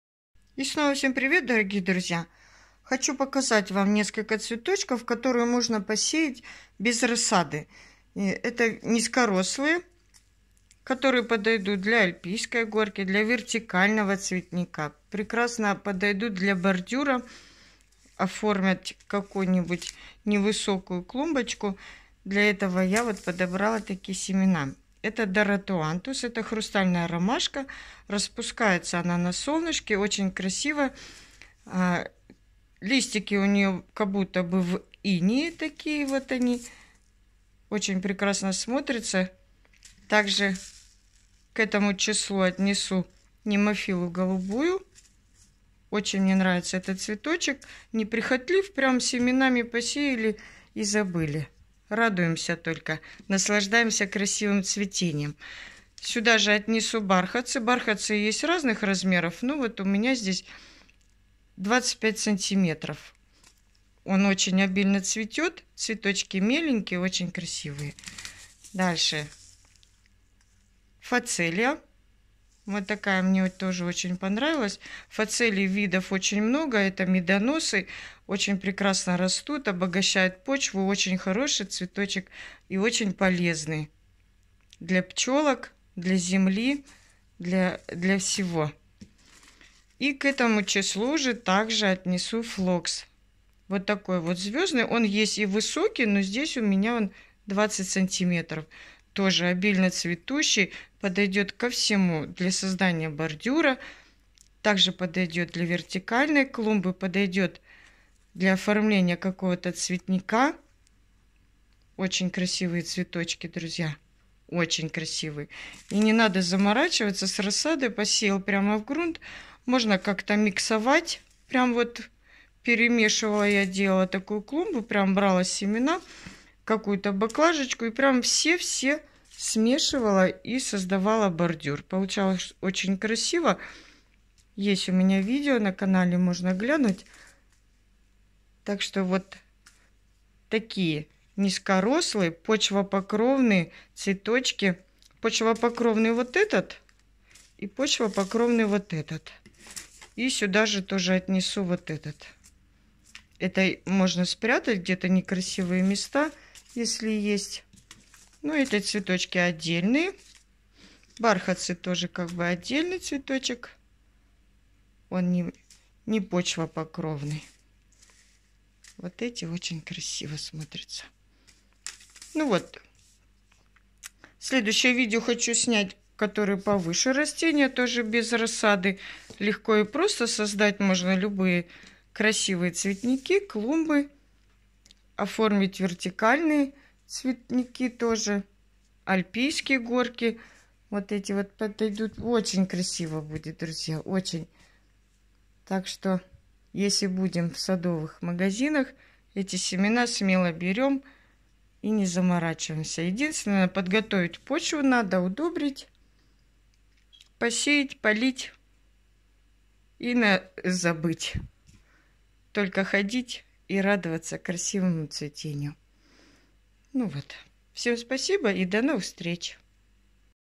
Бордюрные, обильноцветущие, БЕЗ РАССАДНЫЕ ЦВЕТЫ, для альпийских горок, вертикальных клумб, микс
Доротеантус, флокс друмонда, бархатцы, немофила, фацелия, все эти цветочки до 25 см высотой, прекрасно подойдут для создания бордюра, для оформления вертикальных клумб, для красивого низкорослого цветника,для АЛЬПИЙСКОЙ горки, сеем семенами прямо в землю,поливам, они быстро растут, заполняют все пустоты, долго и обильно цветут, есть ещё много цветочков, но вот мне приглянулись такие и я ими полностью довольна и вам они тоже понравятся, удачи:))